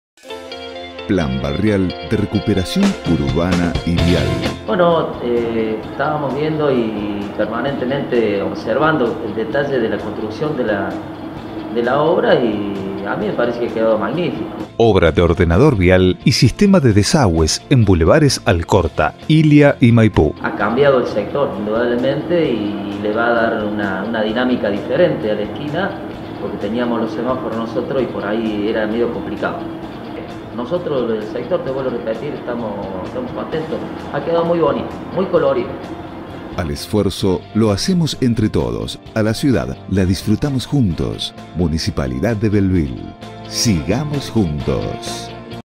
Nuevos desagües pluviales y Ordenador Vial, testimonial - Municipalidad de Bell Ville
En obras públicas seguimos mejorando a Bell Ville, que está en constante crecimiento.

En la unión de los bulevares Alcorta, Illia y Maipú, se solucionó un antiguo problema de anegamiento, con una inversión municipal de ($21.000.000) veintiún millones de pesos.

Al esfuerzo lo hacemos entre todos, a la ciudad la disfrutamos juntos.